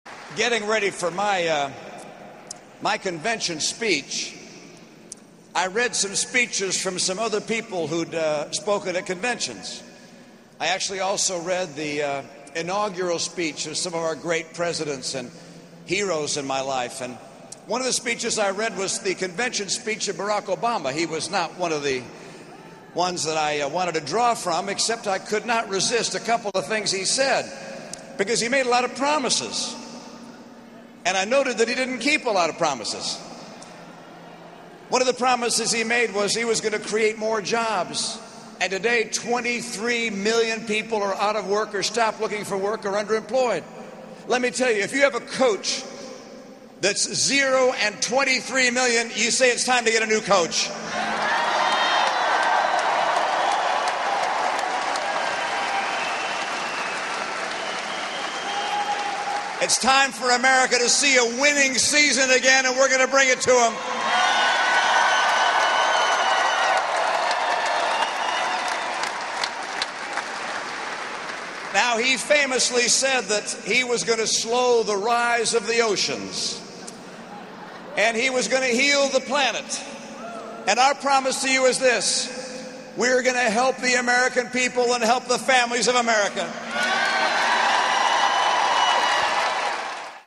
Romney: "Time to Get a New Coach"
Mitt Romney gave a nod to the start of college football season in a speech in Cincinnati, Ohio.  Referring to the unemployed, Romney said, "If you have a coach that's zero and 23 million, you say it's time to get a new coach."  (Sept. 1)